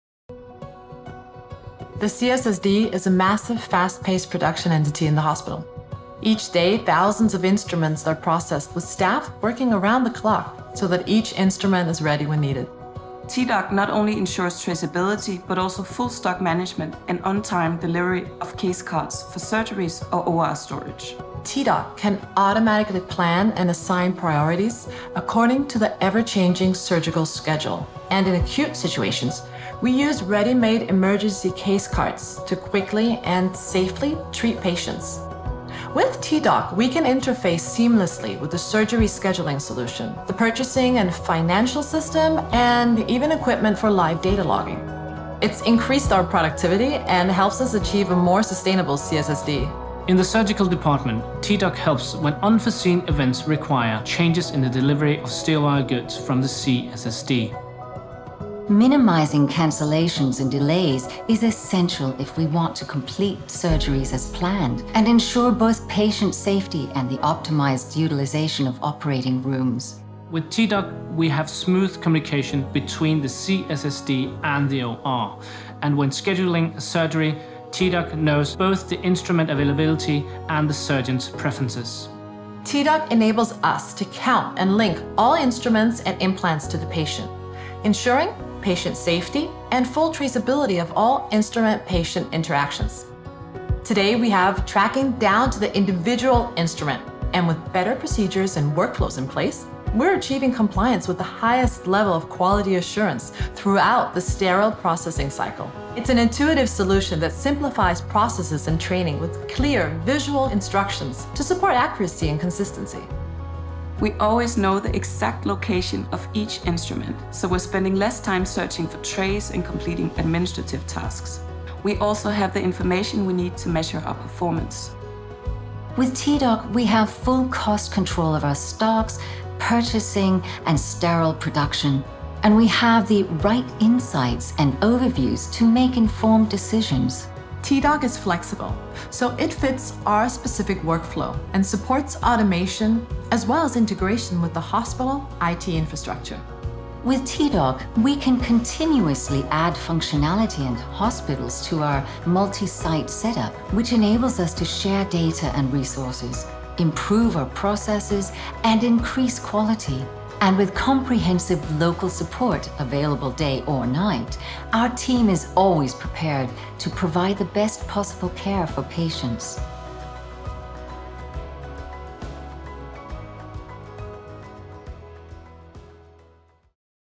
T-DOC 滅菌サプライマネジメントシステム（病院効率化ソリューション）
T-DOCは、すべての器具とインプラントをカウントし、患者と紐付けることができるため、患者の安全、器具と患者のトレーサビリティを確保することができます。T-DOC 2000は、トレーサビリティ、器具や装置の使用率の最適化、手術のオンタイムデリバリーを実現するゲティンゲの滅菌供給管理ソリューションです。これにより、患者に最善の治療を提供することができます。

T-DOC 滅菌サプライマネジメントシステム について